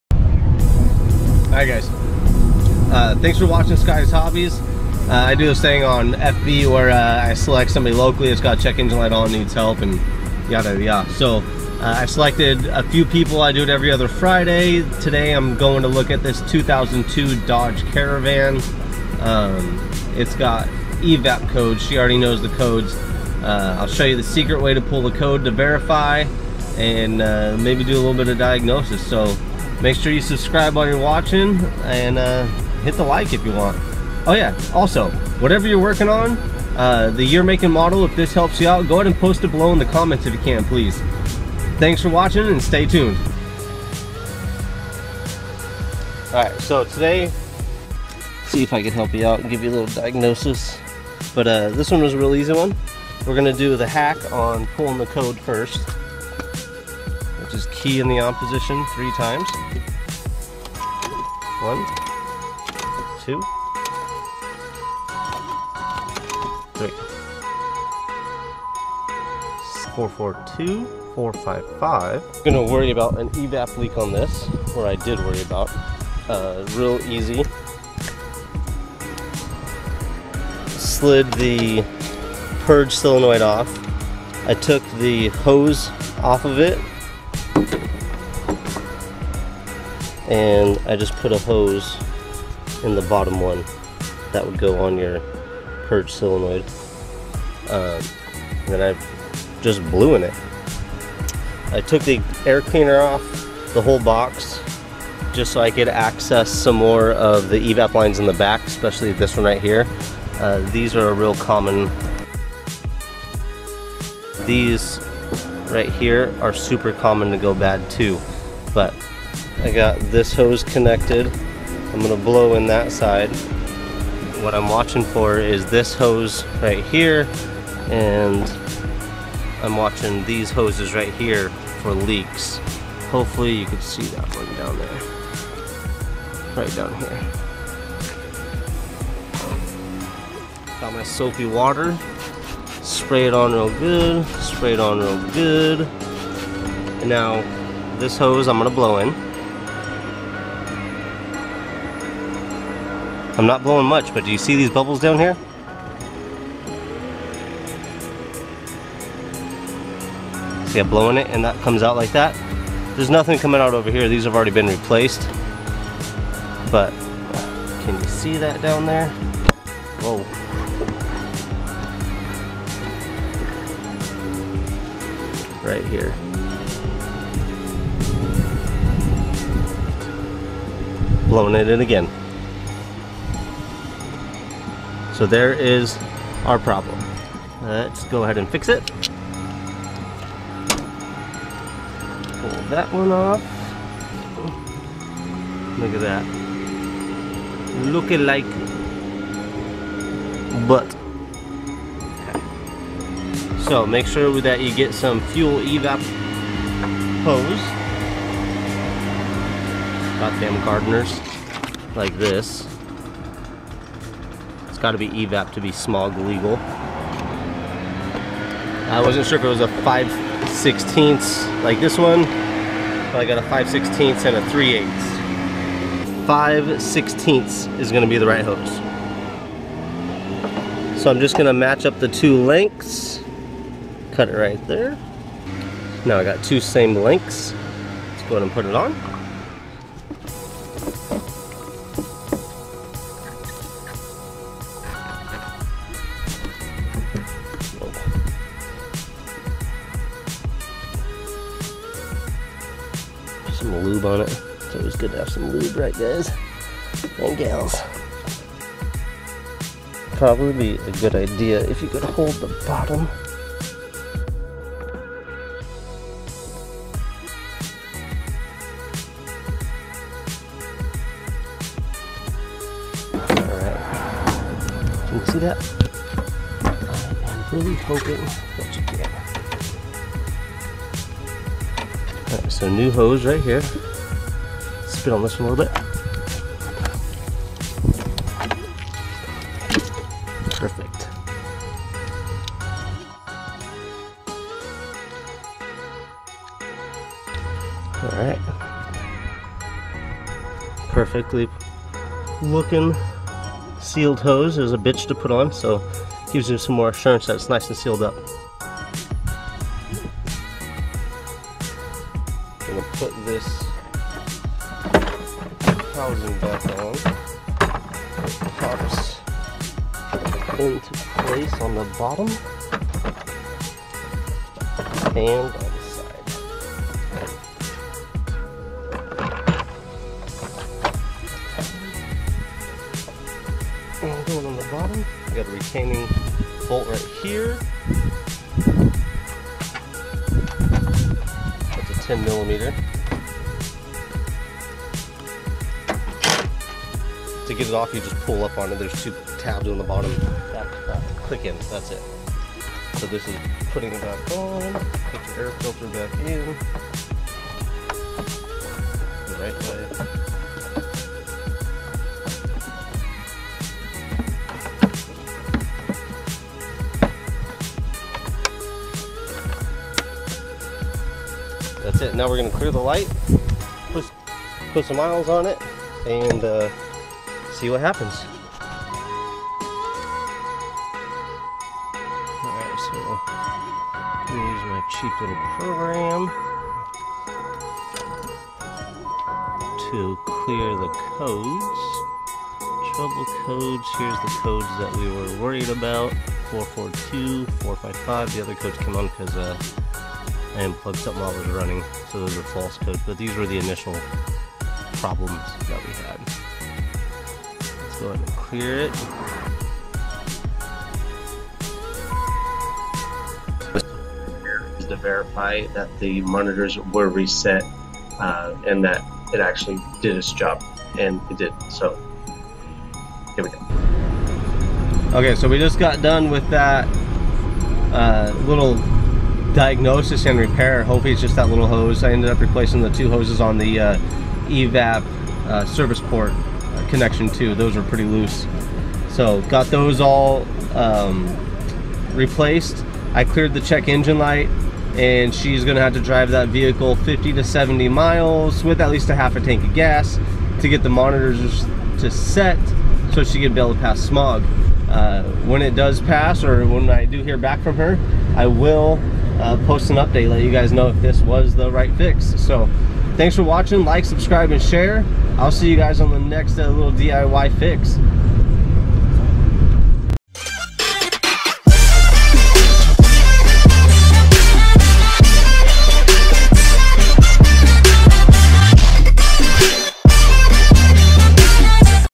OBD code P0442 P0455 P0456 diagnosis and fix. Easy!!!
Follow me along as I take a look at this 2002 Dodge Caravan. Man this whole problem was codes stored in the computer having p0442 and P0455. Video a quick little diagnosis. Using some spare vacuum hose I had laying around and a spray bottle with soapy water in it. Best quick little fix should also be the same as on a Chrysler Town & Country. Also not all EVAP system leaks are going to be the same exactly this is more of a way to start your diagnosis and how I easily fixed this problem.

Want to send us stuff to try:
Scotties Hobbies
2455 N Naglee Rd #143

Facebook.com/TheRealScottiesHobbies

outro credit:
EPICmoh Gaming